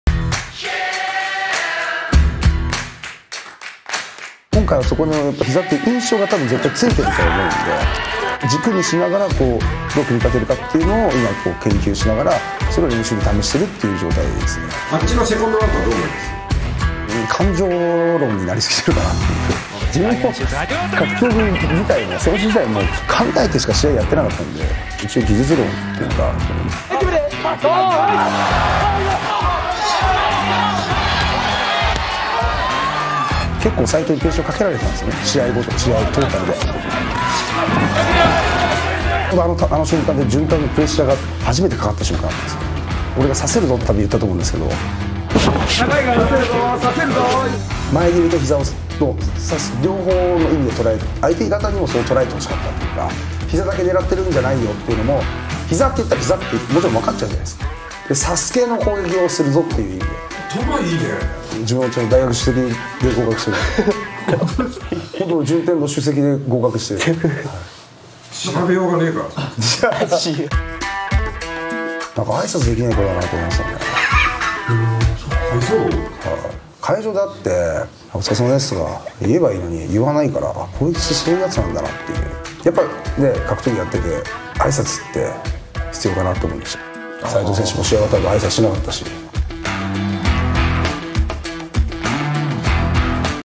”斎藤陣営に挨拶出来ない奴”としゃべりすぎる横田代表に牛久選手この表情
このチャンネルはガジェット通信様を通してRIZIN FIGHTING FEDERATIONチャンネルへ報告済です。
収益後改めて連絡予定です。